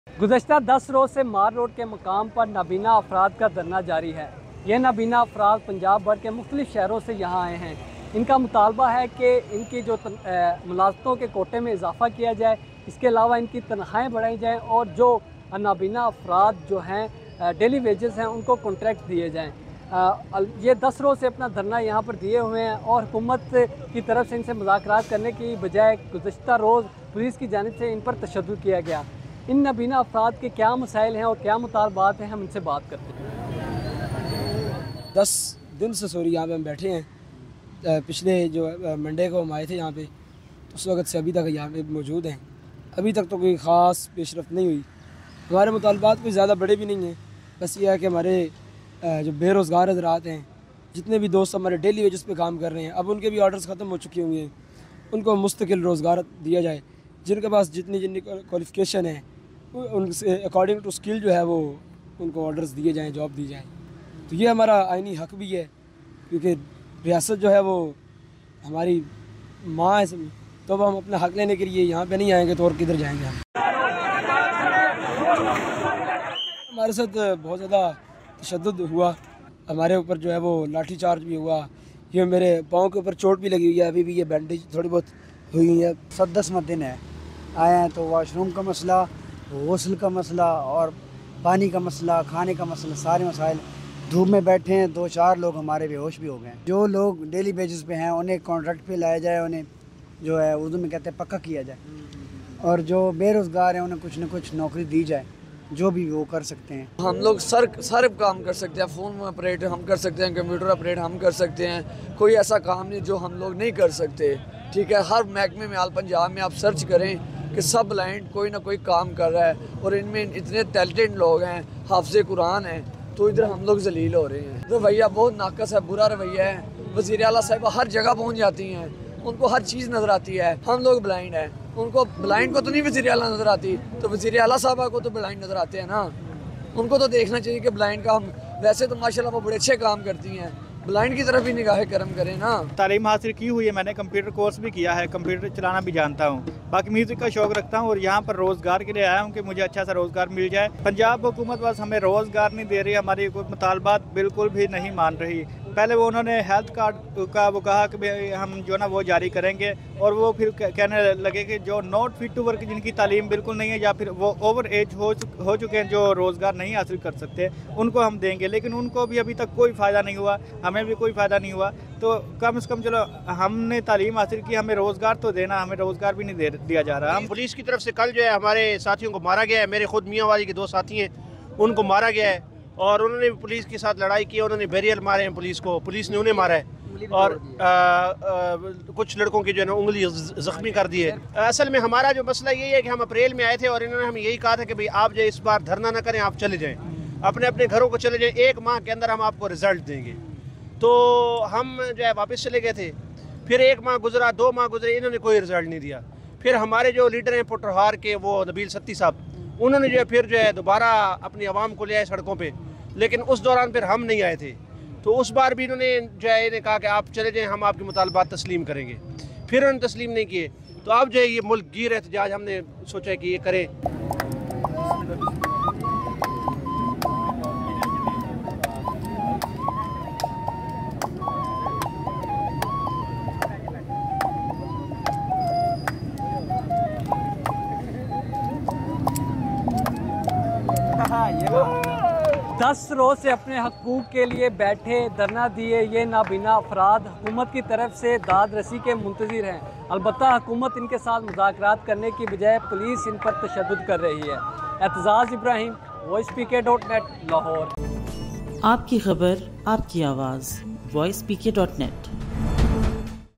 Sit-in by visually impaired persons in Lahore enters into 10th day
لاہور میں نابینا افراد کا احتجاجی دھرنا دس روز سے جاری ہے۔ مظاہرین ملازمتوں میں کوٹے کا اضافہ، کنٹریکٹ ملازمین کی مستقلی، اور تنخواہوں کی بروقت ادائیگی کا مطالبہ کر رہے ہیں۔  یہ دھرنا چیئرنگ کراس پر آٹھ دن جاری رہا، مگر حکومتی عدم توجہ پر گزشتہ روز مظاہرین نے وزیر اعلیٰ ہاؤس کے سامنے دھرنے کا رخ کیا، جہاں پولیس نے مزاحمت کی جس سے کئی نابینا افراد زخمی ہوئے ۔